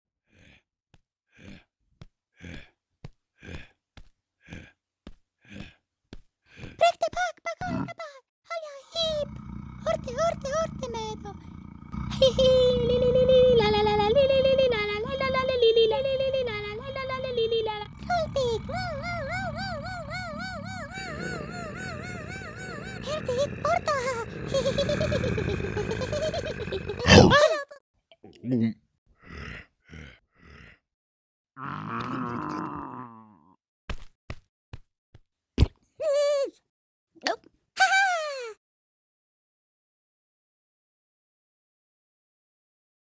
Pixie Iip in: Bearly happy
A short cartoon about a happy little pixie called Iip